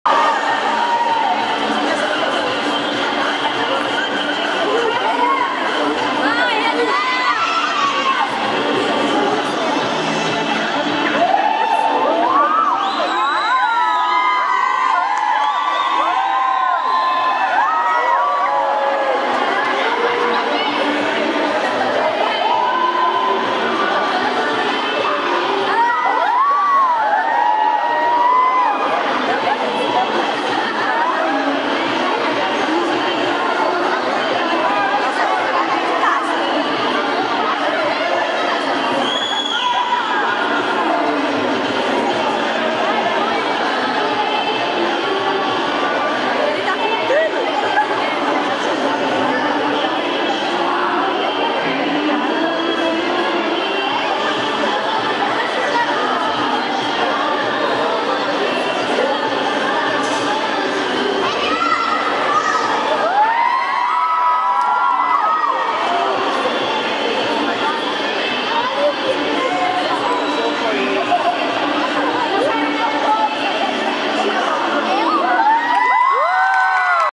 Coreografando o cotidiano - 2013
Trabalho realizado pelos alunos do 9º ano da E.E. Pedro Alexandrino.
Professora Nicole Campanella